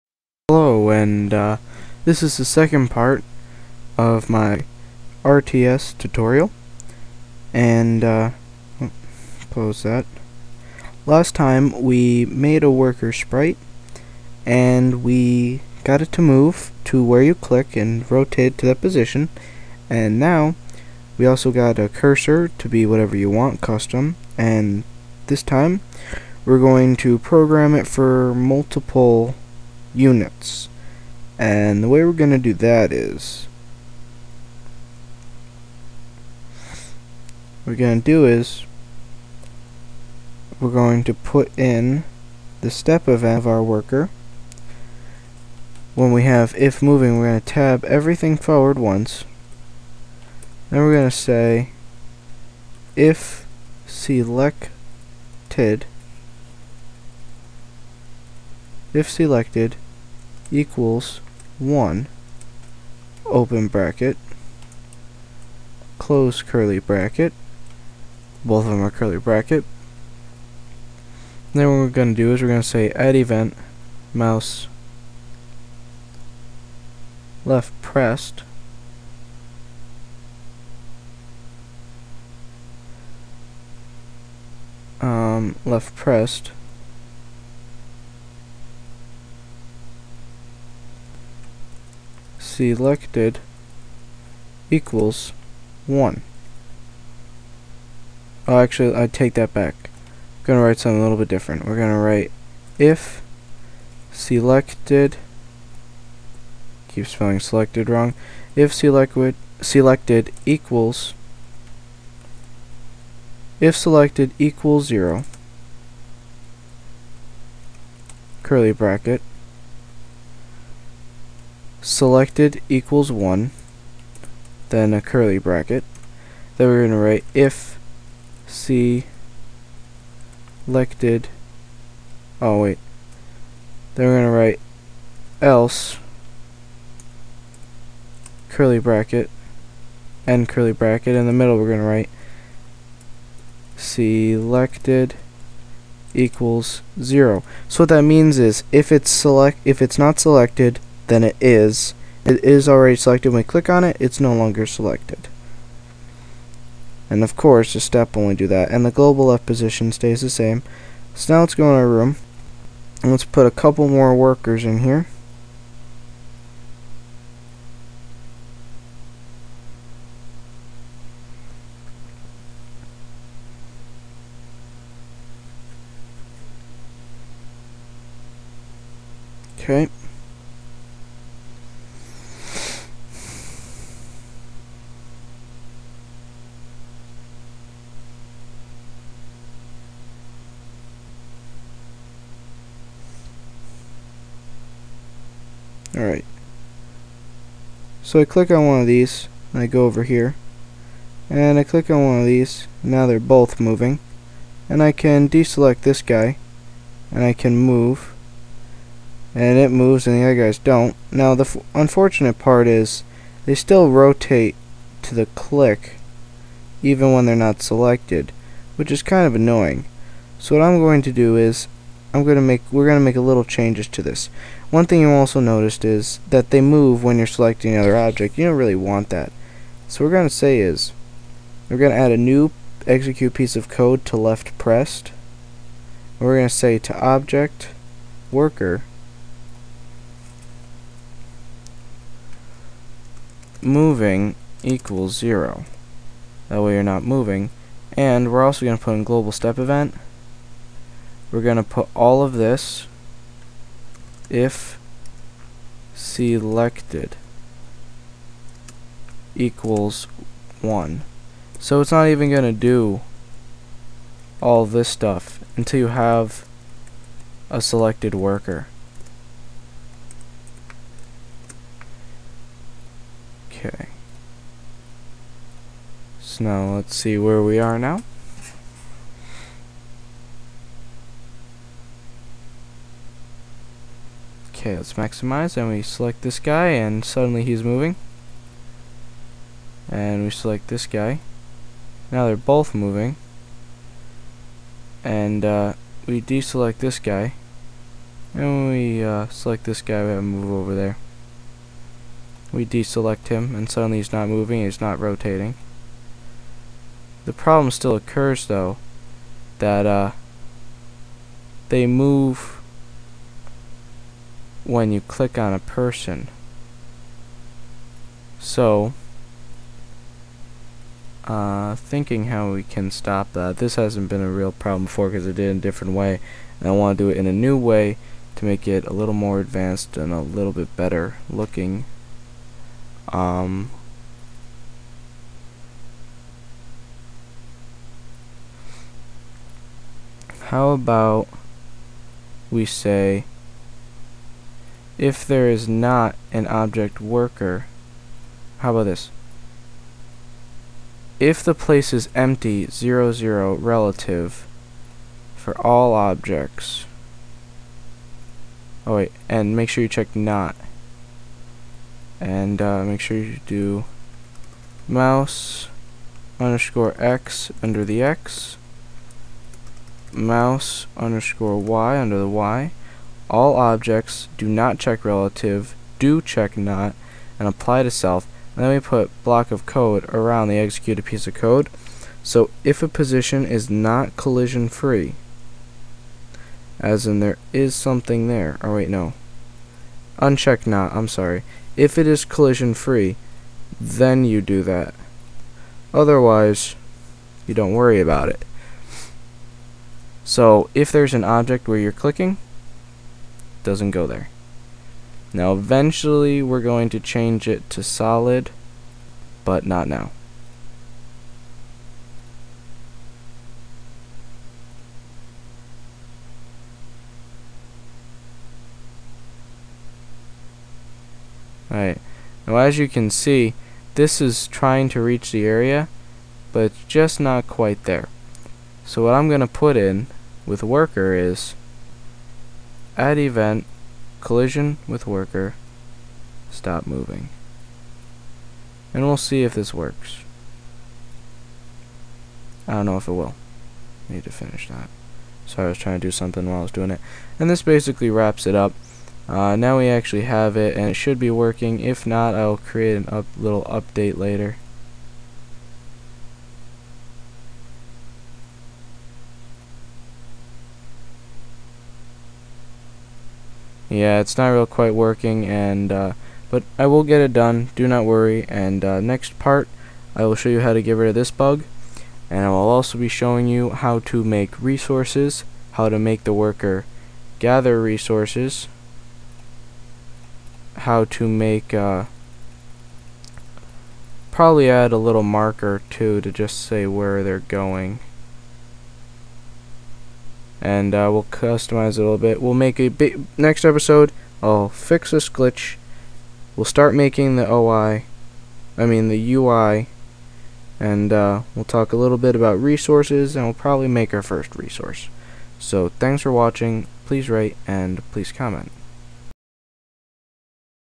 Game Maker Make an RTS Part 2
Part two of the series, we make it so you can select more than one worker and move only selected workers alllowing you to manage the players on the screen.